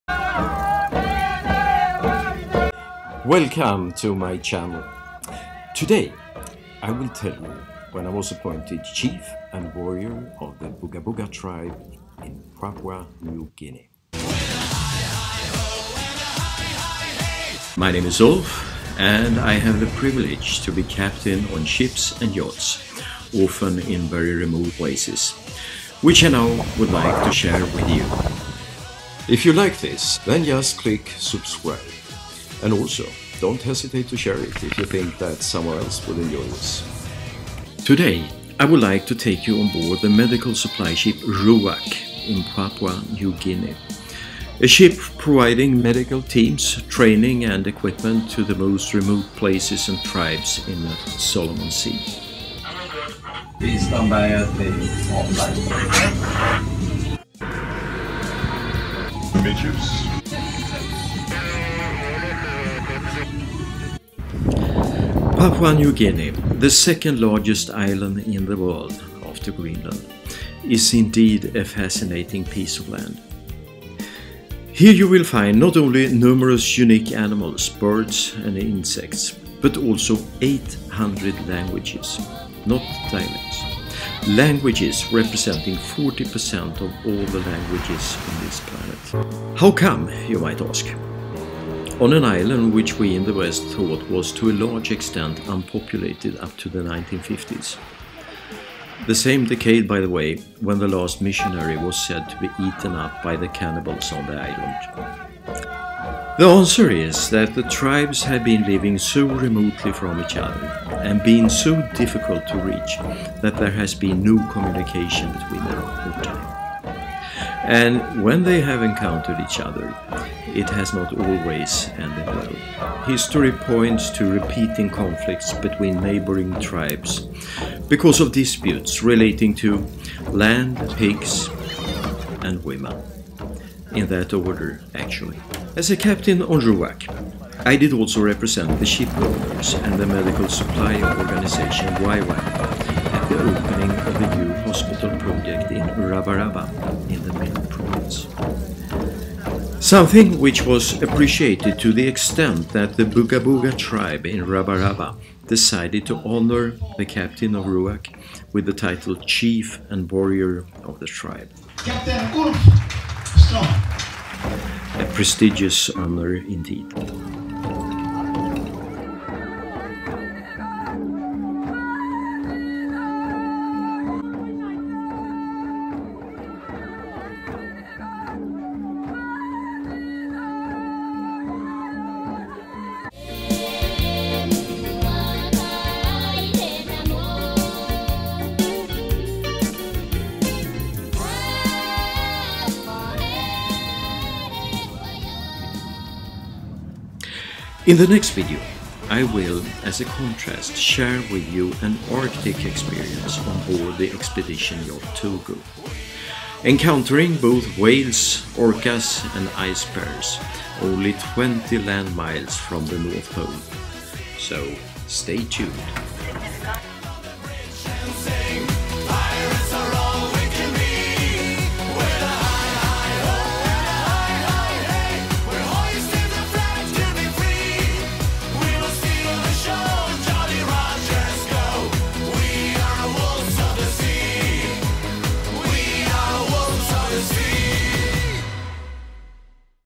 Vlog #1 - A hidden secret in Papua New Guinea
Vlog 1 - Onboard the Medical Supply Ship "Ruach" in Papua New Guinea where I will  disclose how I got honoured with the distinction as Chief and Warrior by the cannibal descendants of the Booga-Booga tribe in Rabaraba.

Map - tourist - adventure - tribes - cannibals - visit - travel - unique - explore - experience - dream - nature - rain forest -  birds - jungle - animals